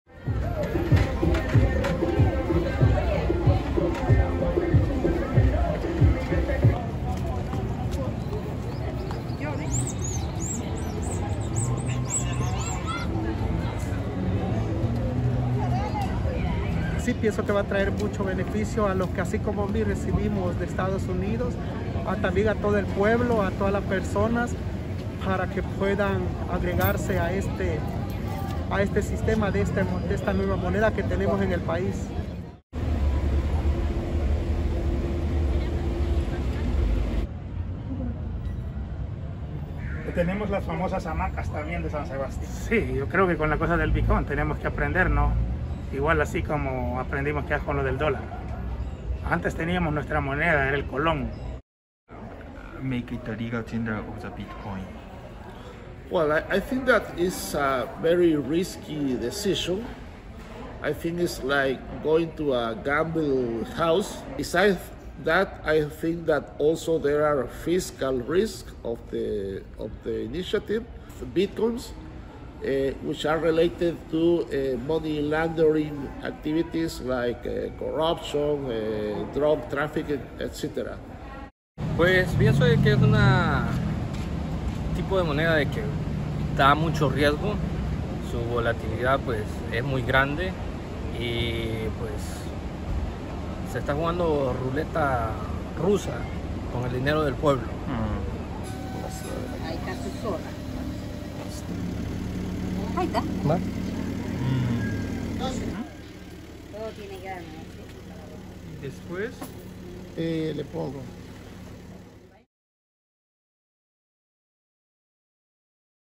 エルサルバドル、ビットコインを法定通貨に 現地ルポ
中米エルサルバドルで７日、暗号資産（仮想通貨）のビットコインが法律で定めた公式の通貨となる。ビットコインの「法定通貨」への採用は世界で初めてだ。準備が進む街の様子を記者が追った。

※日本経済新聞社の動画について改ざんや、許可なく商用・営利 目的で利用することを禁じます。ビットコイン　エルサルバドル